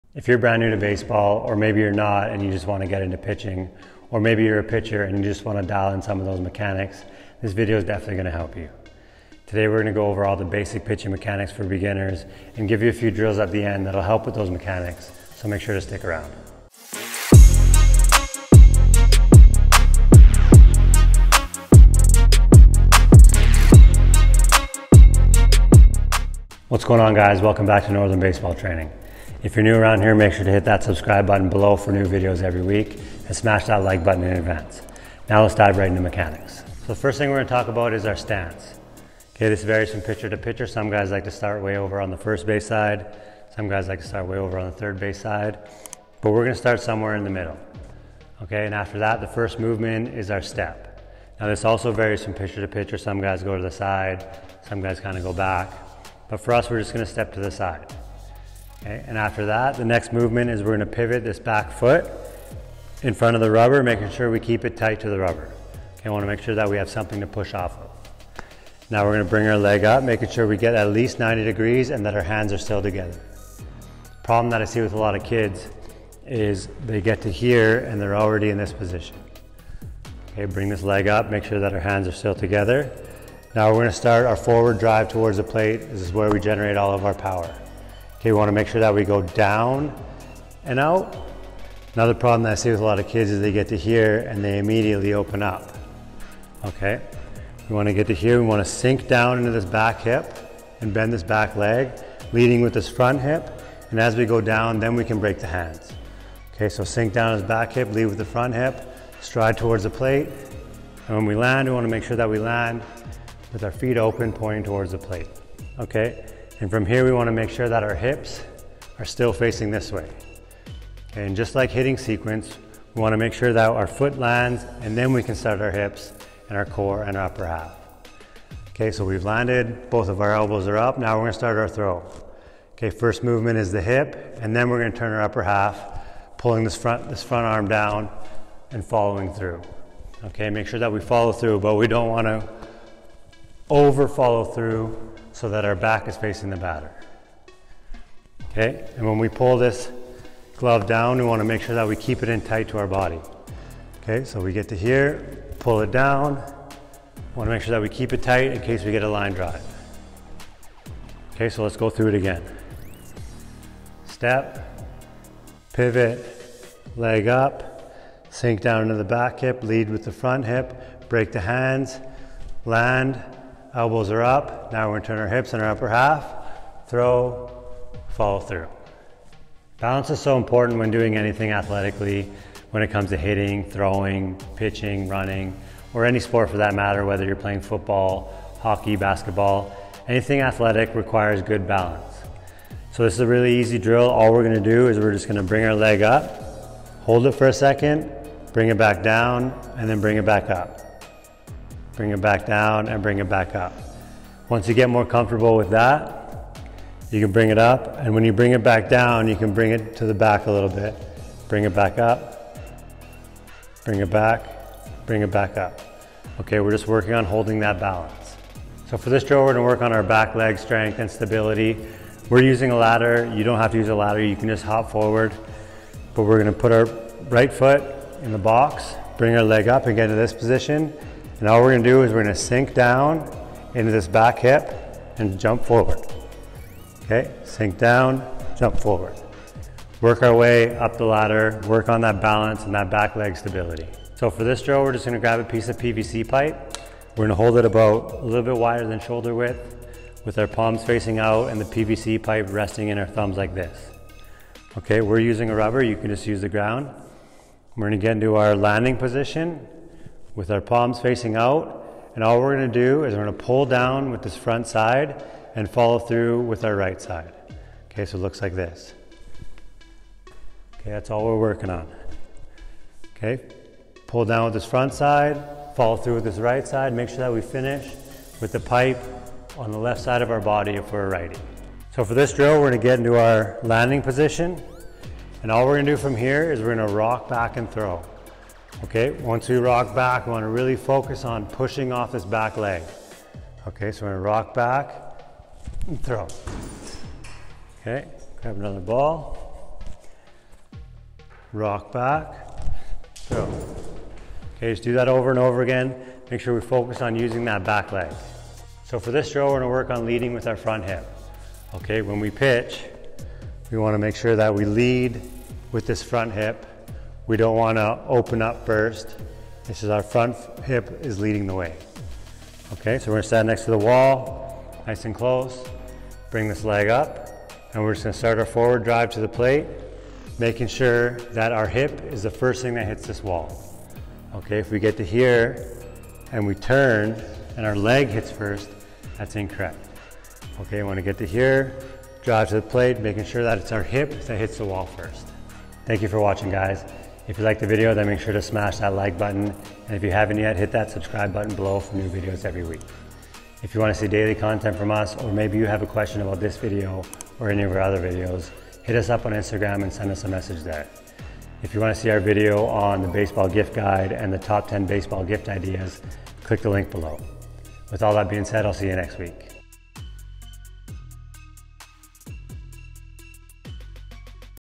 5 EASY Beginner Pitching Drills - Baseball Pitching Mechanics For Youth Players - How To Pitch
In today's video, we will teach you how to pitch and all the basic pitching mechanics for youth players.  And at the end of the video, we have 5 pitching drills that will benefit any level of pitcher.

Whether you are new to baseball and want to learn the basic pitching mechanics or you're already a pitcher and want to brush up on your mechanics, this is the video for you.

0:00 Intro
0:26 Welcome
0:36 Pitching Mechanics
2:59 Drill #1
3:38 Drill #2
4:04 Drill #3
4:41 Drill #4
5:08 Drill #5
5:46 Outro

The #1 Hitting Tool!👇

Best Bands For Arm Workouts👇